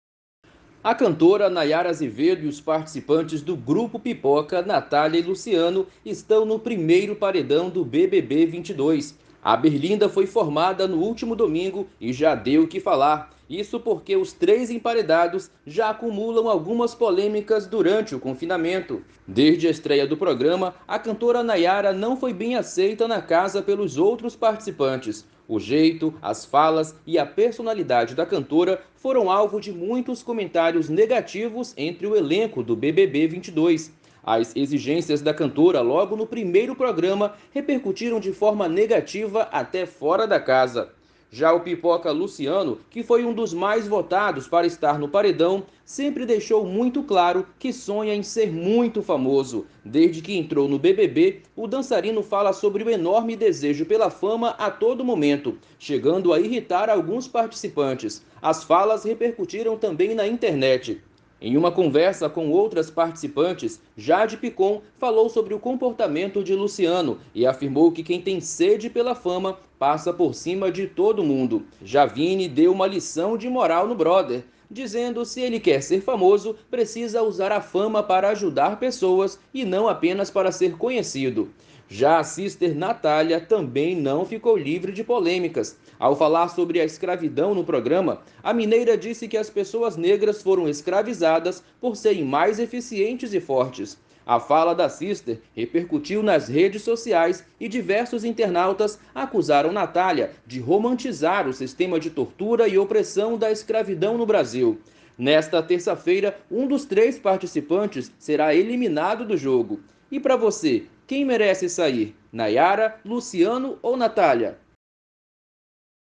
Polêmicas: Quem deve sair do BBB 22?
A cantora Naiara Azevedo e os participantes do grupo pipoca Natália e Luciano  estão no primeiro paredão do BBB 22. A berlinda foi formada no último domingo e já deu o que falar, isso porque os três emparedados já acumulam algumas polêmicas durante o confinamento.